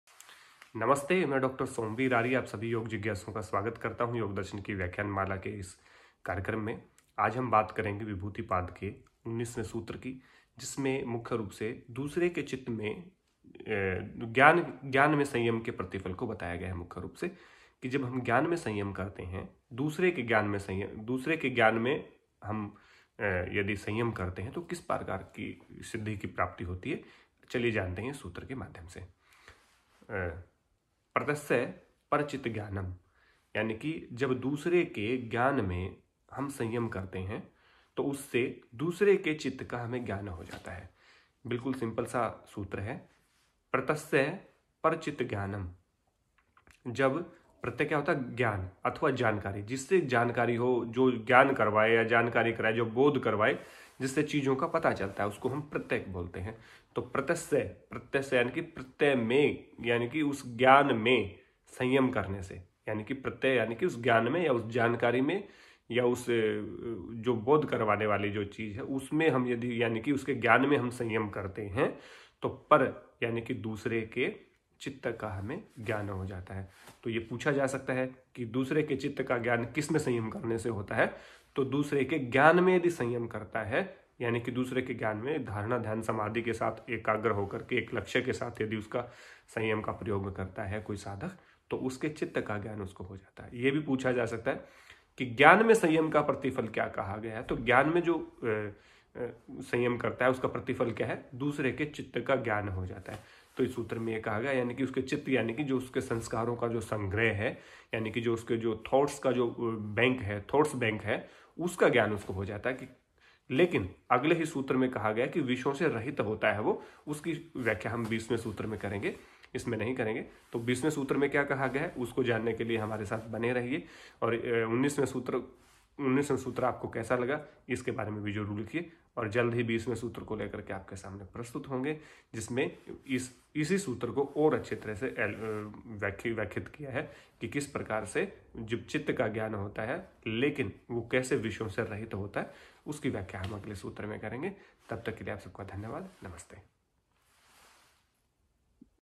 योगदर्शन विभूतिपाद सूत्र-19 Yogadarshana 3/19 दूसरे_के_ज्ञान_में_संयम _का_फल
दूसरे के ज्ञान में संयम करने से मिलने वाले फल को जानने के लिए इस व्याख्यान को सुने ।